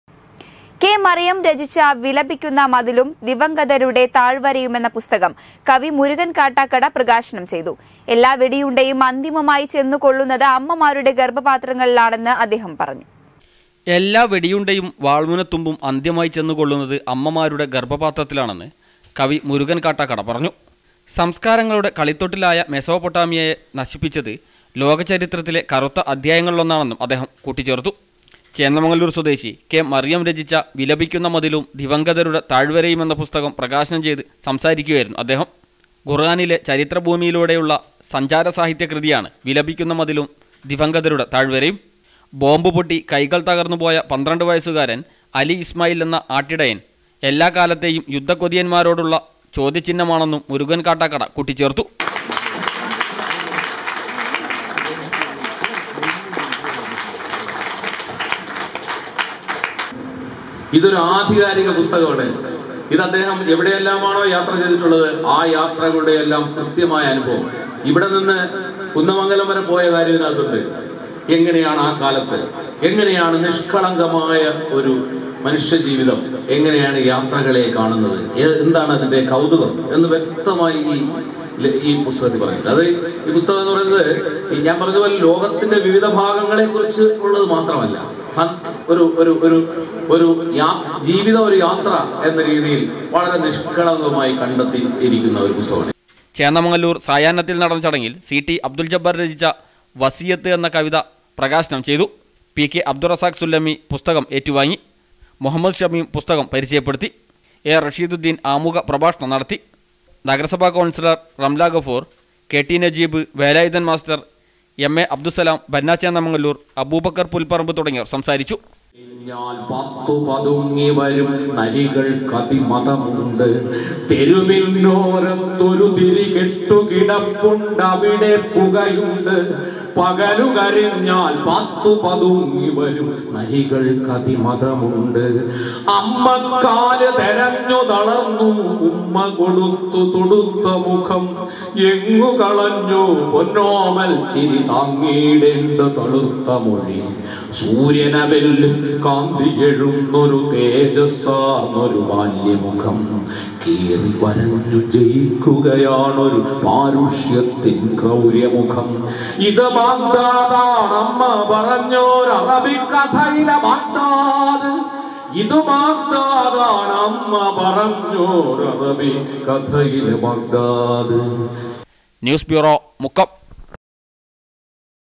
കെ. മറിയം രചിച്ച വിലപിക്കുന്ന മതിലും ദിവംഗതരുടെ താഴ്‌വരയും എന്ന പുസ്തകം കവി മുരുകൻ കാട്ടാക്കട
കെ. മറിയം രചിച്ച വിലപിക്കുന്ന മതിലും ദിവംഗതരുടെ താഴ്‌വരയും എന്ന പുസ്തകം കവി മുരുകൻ കാട്ടാക്കട പ്രകാശനം ചെയ്തു. എല്ലാവെടിയുണ്ടയും അന്തിമമായി ചെന്നുകൊള്ളുന്നത് അമ്മമാരുടെ  ഗർഭപാത്രങ്ങളിലാണെന്ന് അദ്ദേഹം പറഞ്ഞു.